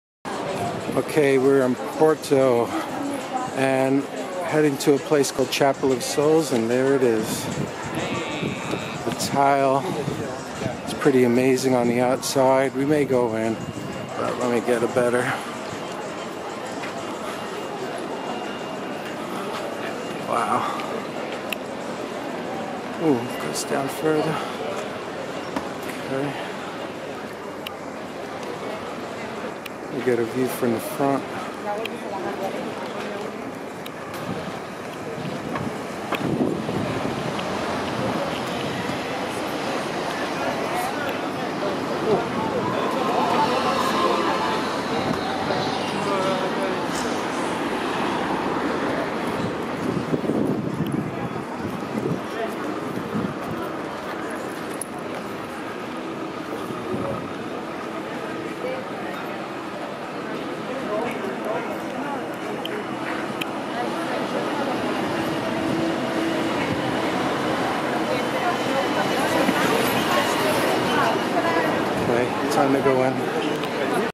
Walking to Chapel of Souls in Porto, Portugal (HD)
Covered in blue painted tiles, this unique chapel located in the very heart of Old Porto is a must see. There are 15,947 tiles in total covering 360 square meters. The pictures made of these tiles depict the death of St Francis, the martyrdom of St Catherine and other scenes from the lives of the saints.